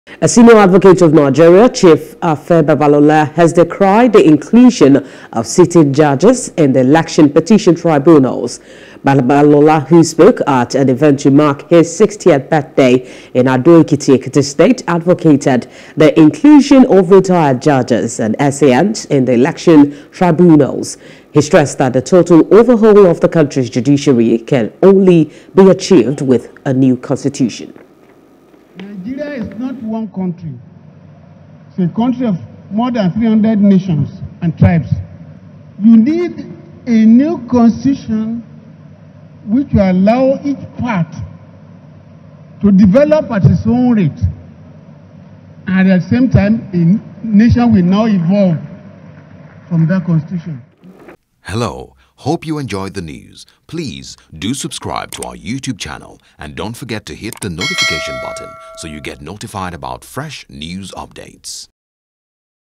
Election Tribunal: Afe Babalola Decries Inclusion Of Sitting Judges
A Senior Advocate of Nigeria (SAN), Chief Afe Babalola, decries the inclusion of sitting judges in the election petition tribunals.